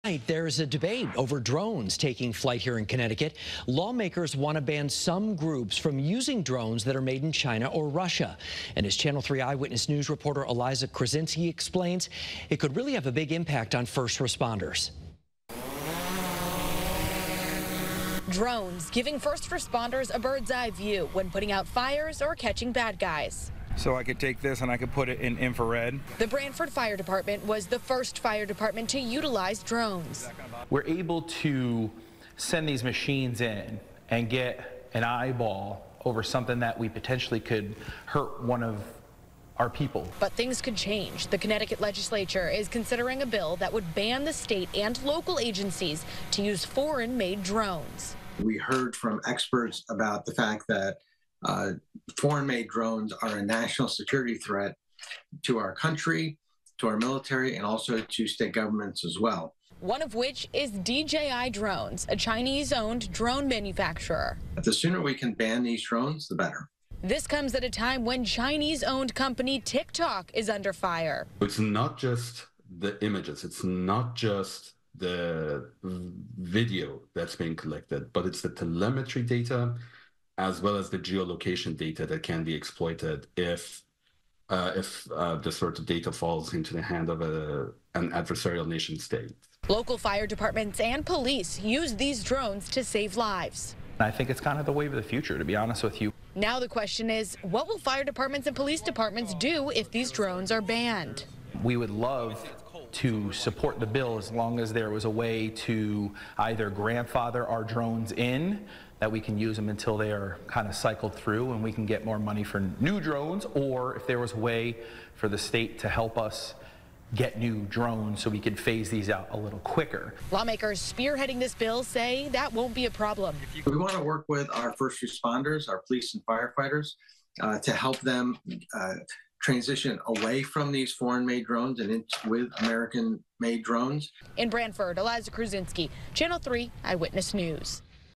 Bill could ban use of foreign made drones
The use of foreign made drones could come to an end in Connecticut, but many towns and cities use these drones to help with public safety.